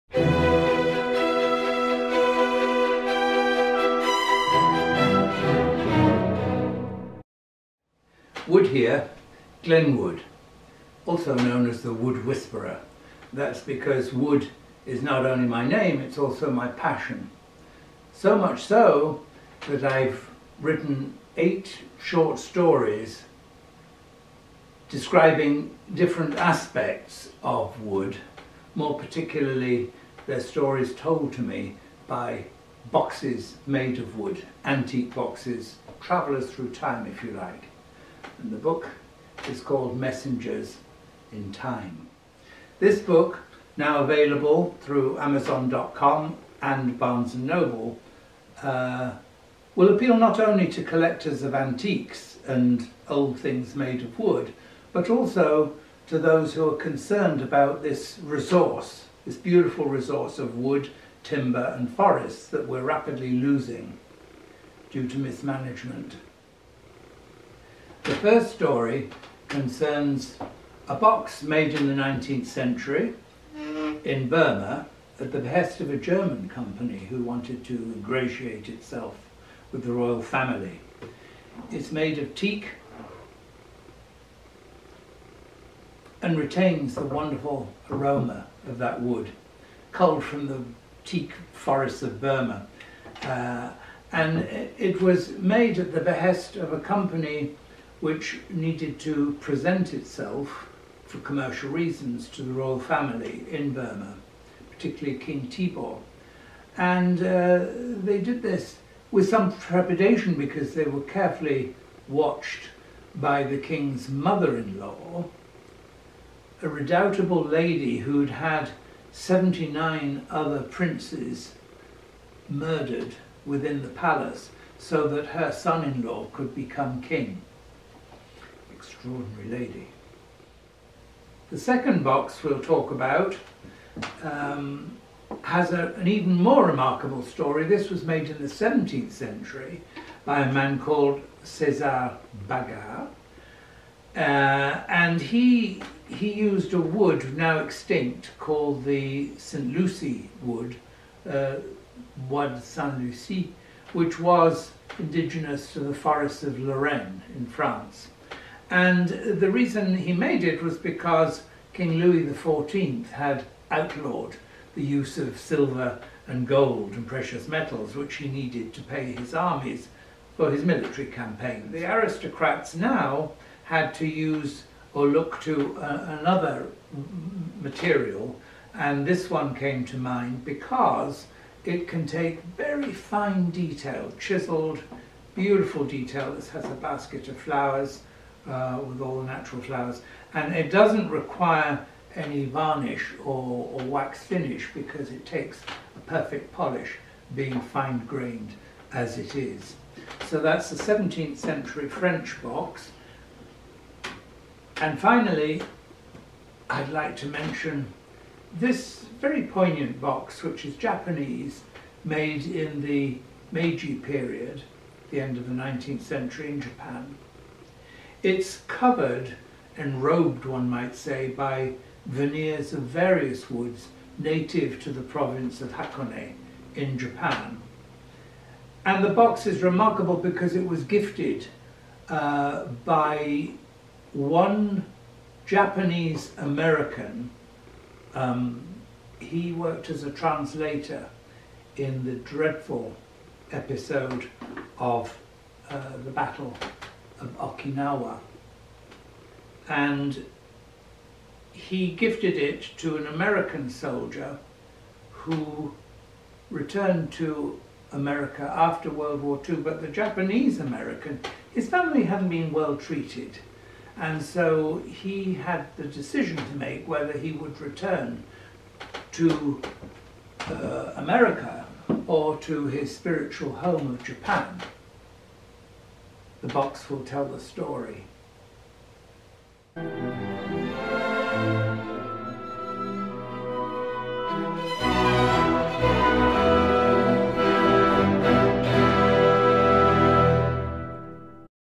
Messengers In Time
'Messengers In Time' contains the accounts of eight historical wooden boxes. The short stories entertain and inform how the boxes were made, why they were made and how they survived the centuries to come to rest in USA. The stories were related to Dr Wood by the boxes themselves and so the information contained in them can be considered reliable. ;)